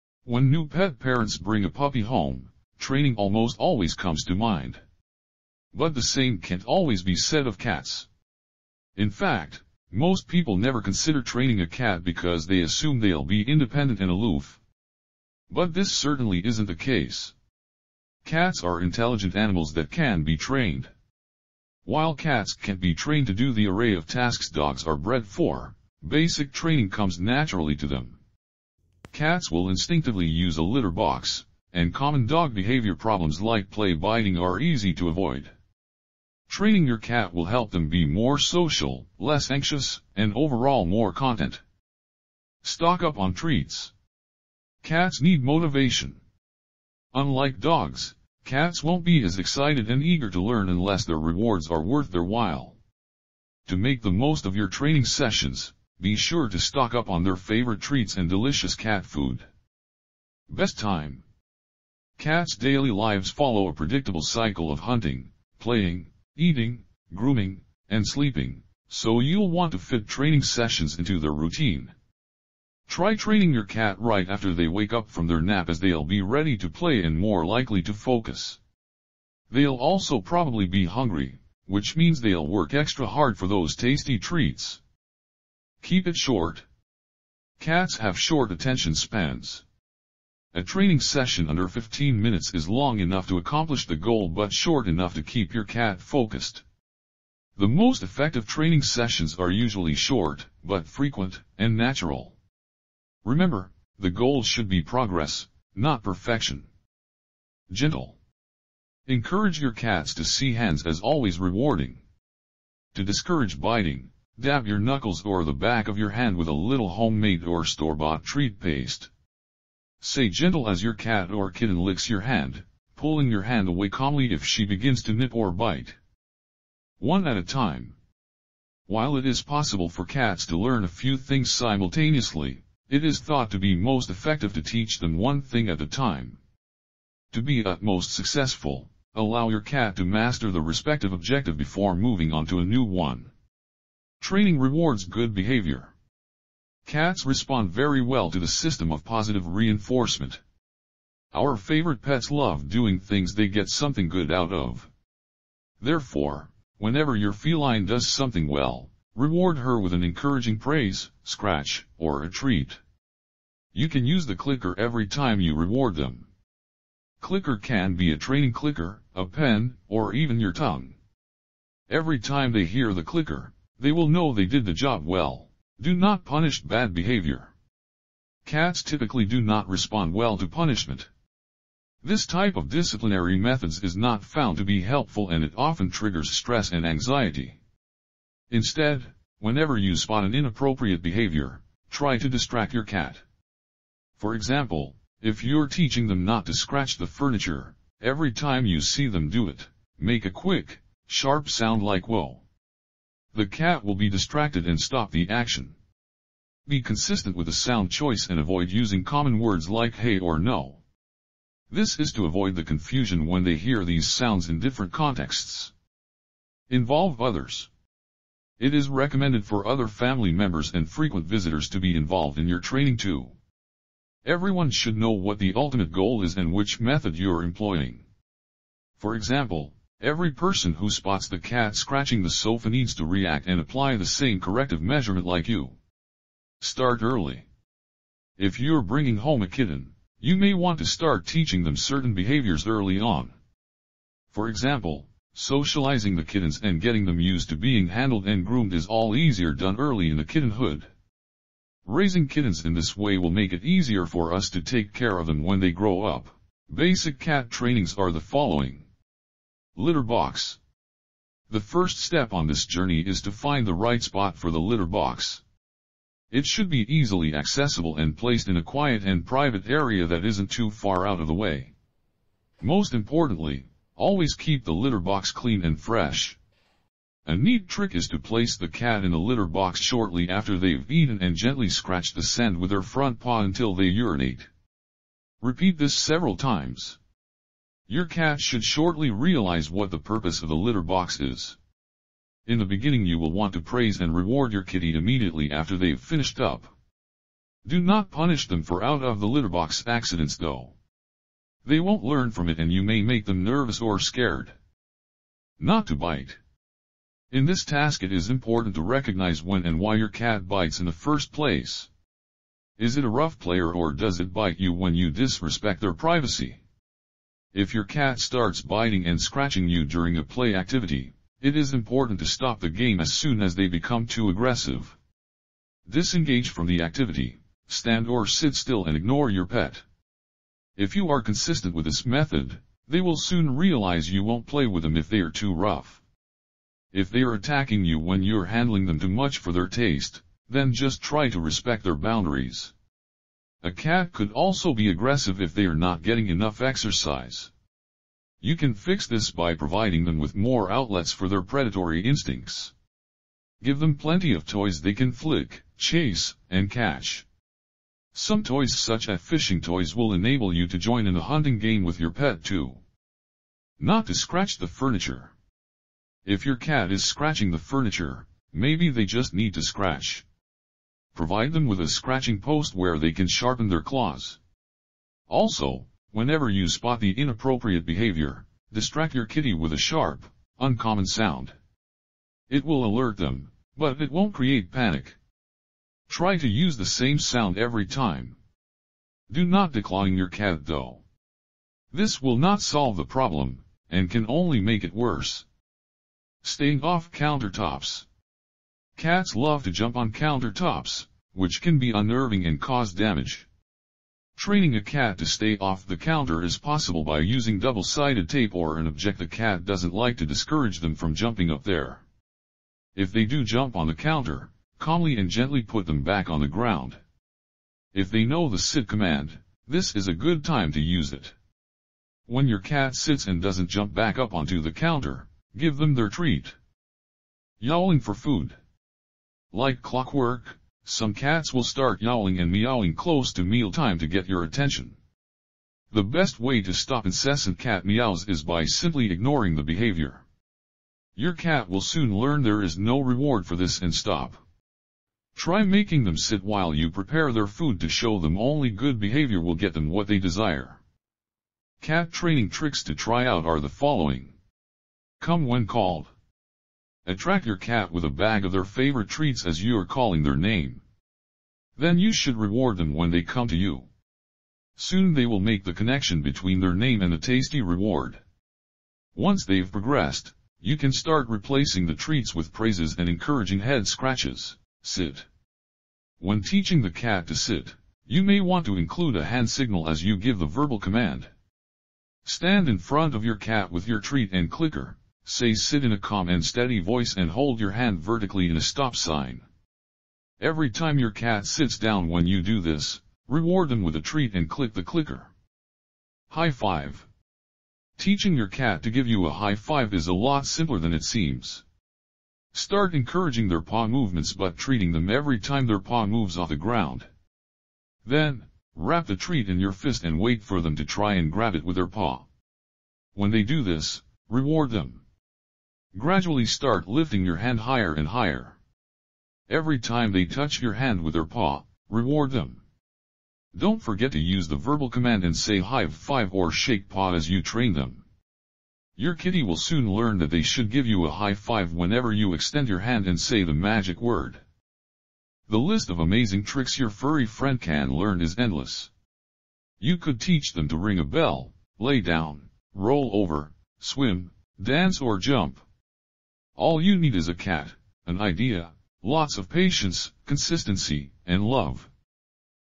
How to train a cat. Cat training tricks.
How to train a cat.  Cat training tricks.  Bound with your cat.  Grow with your cat.

Timestamps:

00:00  Intro
00:42  Stock treats
01:00  Best time
01:25  Keep it short
01:47  Gentle
02:07  One at a time
02:24  Rewards good behavior
02:55  Do not punish bad behavior
03:37  Involve others
03:56  Start early
04:17  Basic cat trainings
04:19  Litter box
05:05  Not to bite
06:03  Not to scratch the furniture
06:36  Staying off counter tops
07:07  Yowling for food
07:31  Cat Training Tricks
07:35  Come when called
07:55  Sit
08:16  High five
08:55  Summary